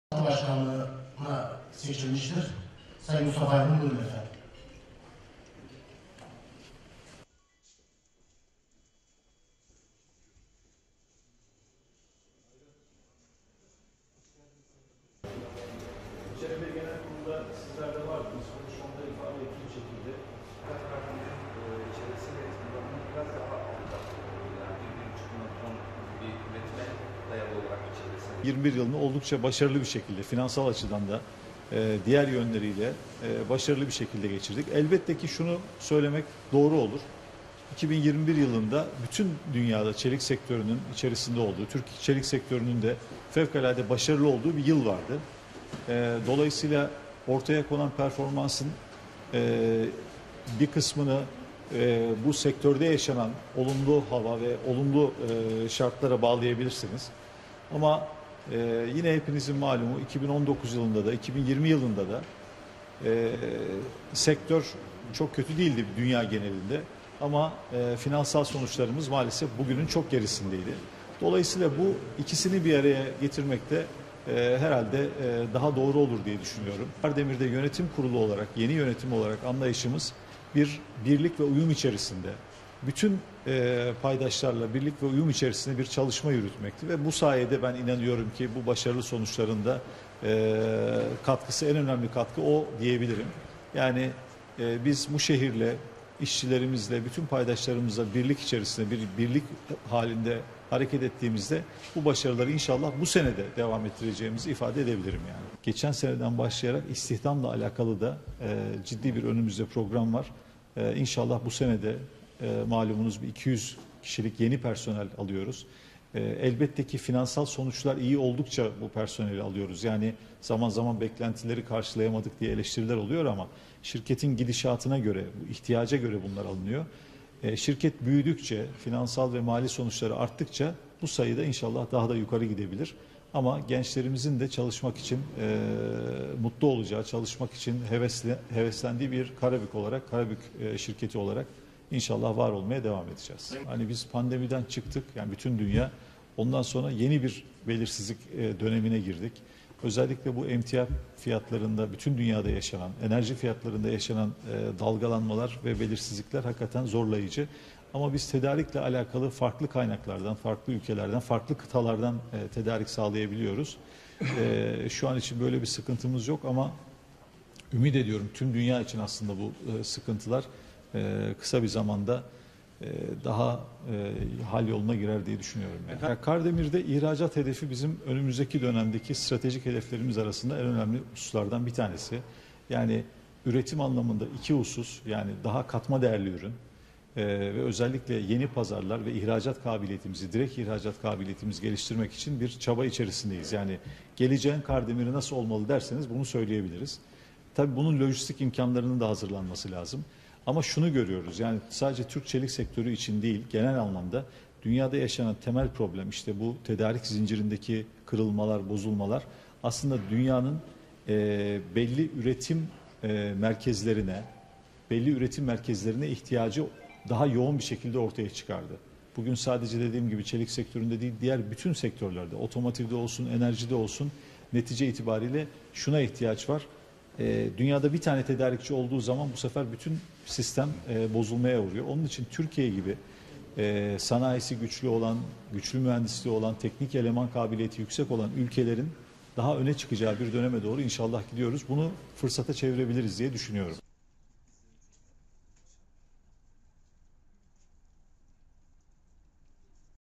Bayraktar, genel kurul sonrası gazetecilerin sorularını cevapladı
KARDEMİR Yönetim Kurulu Başkanı Dr. Alparslan Bayraktar, dünyada bir tane tedarikçinin olmasının bütün sistemi bozduğunu, Türkiye gibi sanayisi güçlü, teknik eleman kabiliyeti yüksek olan ülkelerin daha da öne çıkacağı döneme doğru gidildiğini belirtti.